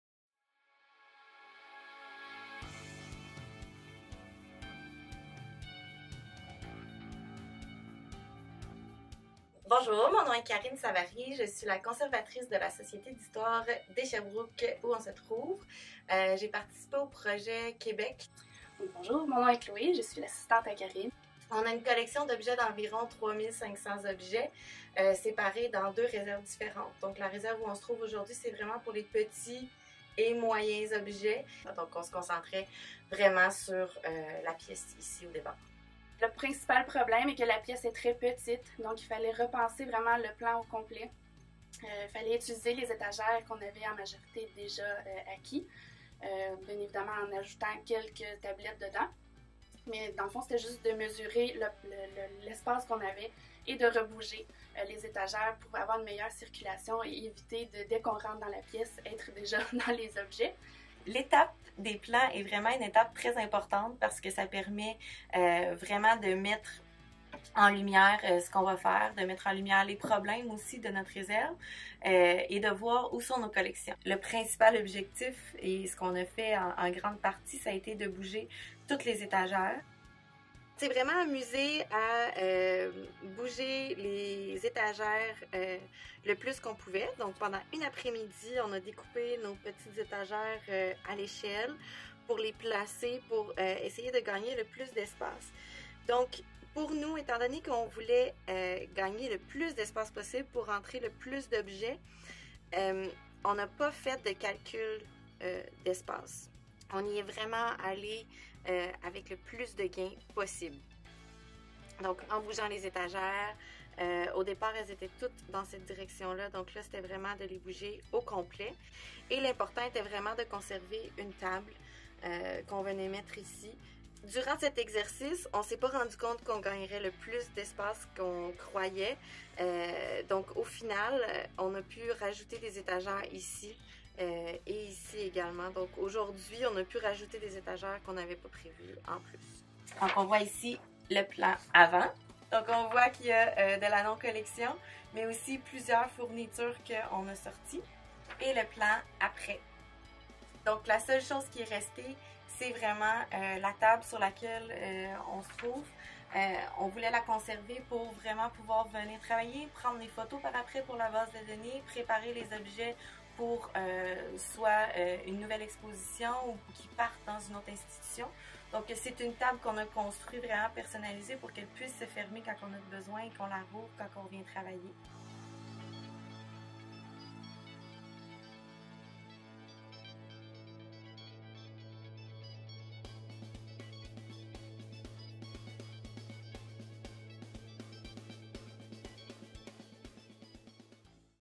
Using floor plans to optimize storage space (RE-ORG Canada)
A tutorial video produced by the Canadian Conservation Institute (CCI) that illustrates how the Sherbrooke Historical Society (participants in the RE-ORG Canada [Quebec] program) used floor plans to optimize space in their small collections storage area.